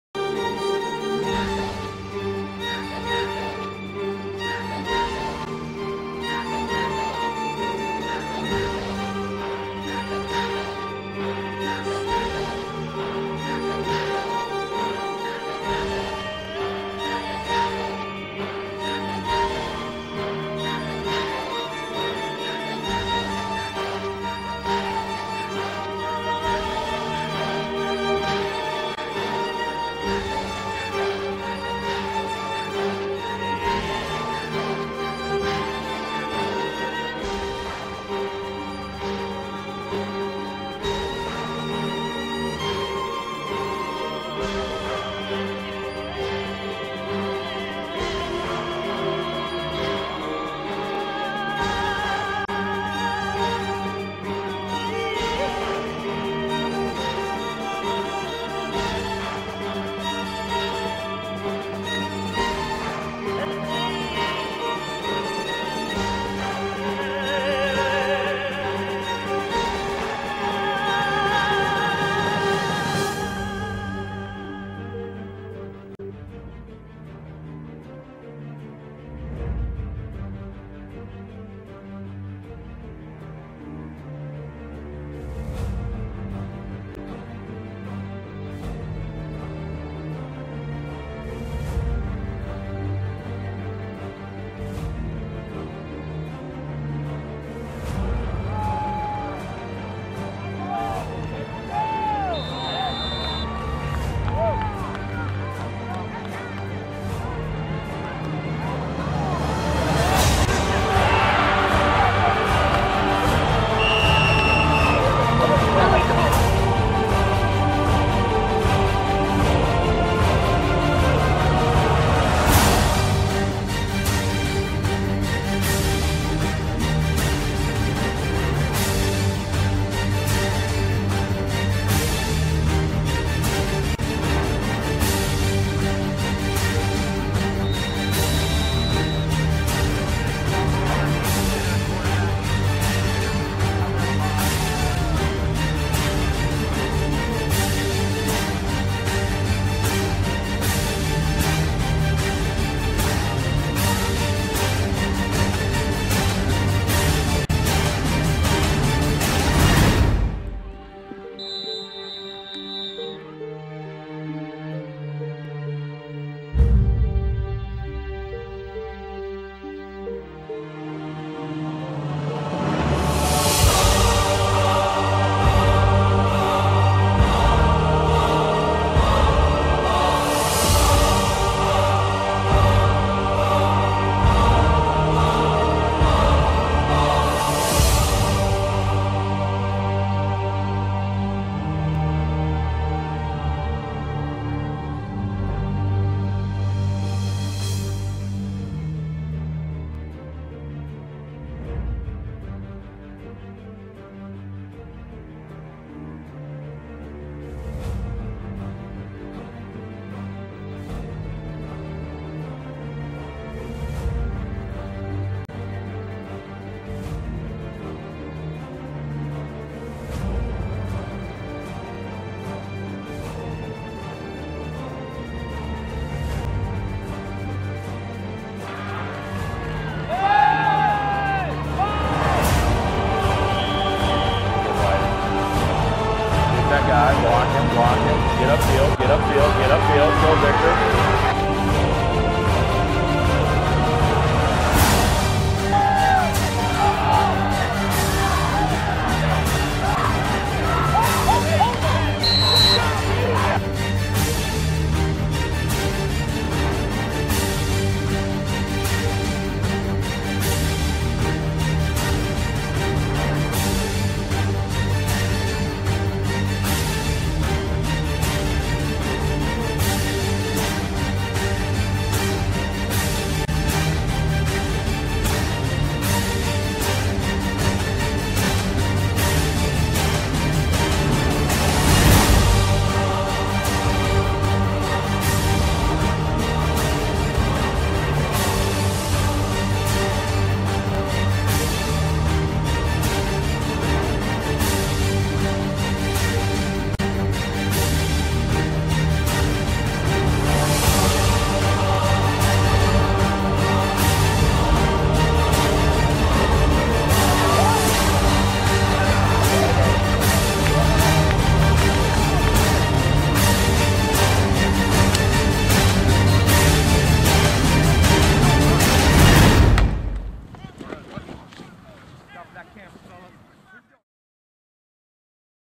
More Westview Wolverine Football
So here is a litle more video that I've captured. Trust me when I say I have more!!! I know some parents were wondering how much video of their son I would capture and here's a small glimpse. If you ARE or ARE NOT going to buy one of the DVD's please let me know ASAP so I make sure to get even more footage of your son or I don't have to worry about getting more... Thanks,